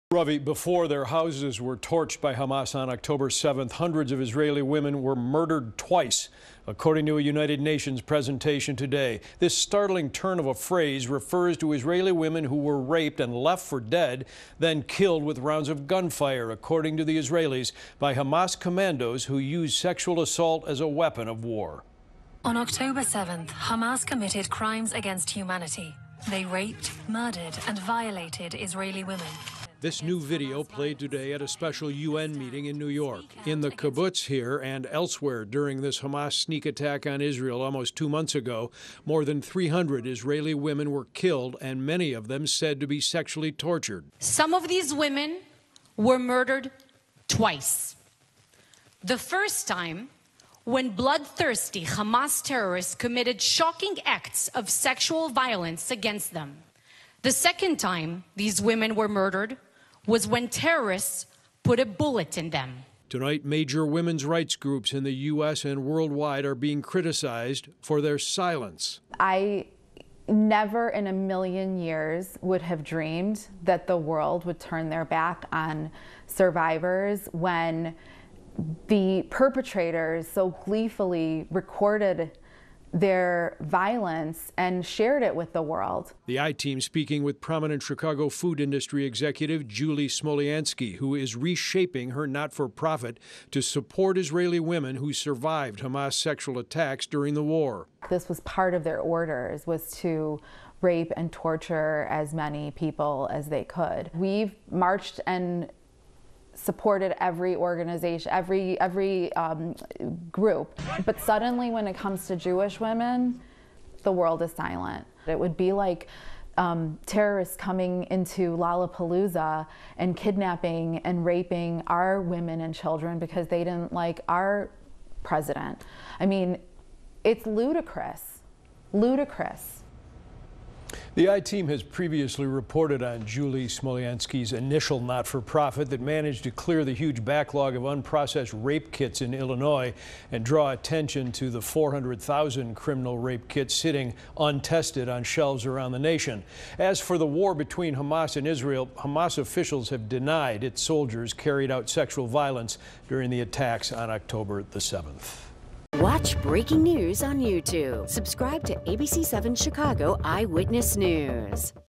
Hamas accused of using rape as weapon of war in Israel
Of the 1,200 people killed during the Hamas attack on Oct. 7, more than 300 Israeli women were killed, and many of those victims were sexually tortured, Israeli forces have said. FULL STORY